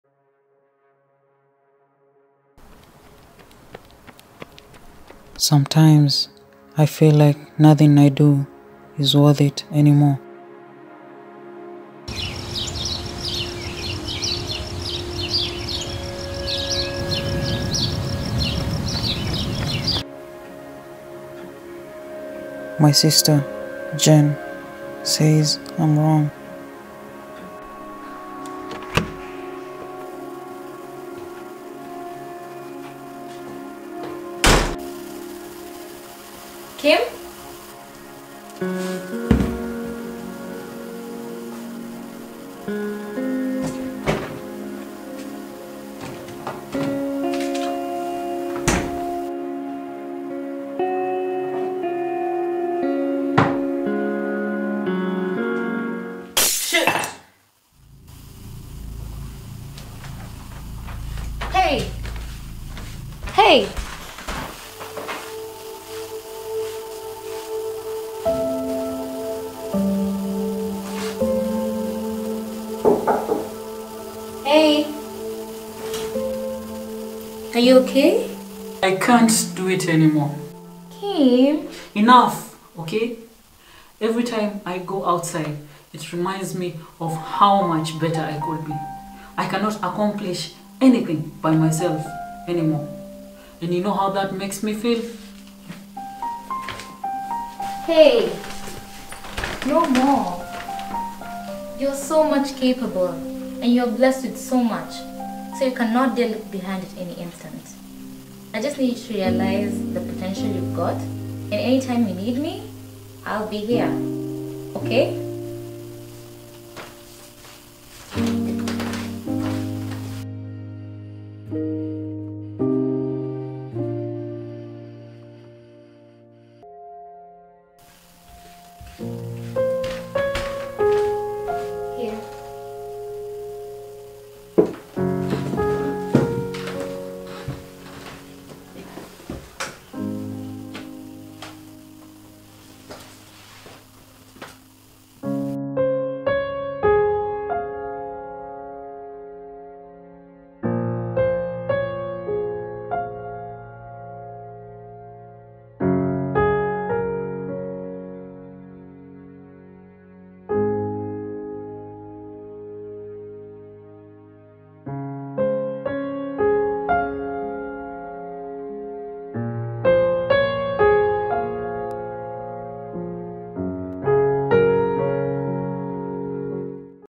amputee (short film)
Well, just a creative short film encouraging all people with disabilities and as well as those facing challenges in life that they suppose they cannot handle. I have my great friends, Jedi and Cooughan to thank for their character role and assistance. Without them I could not manage.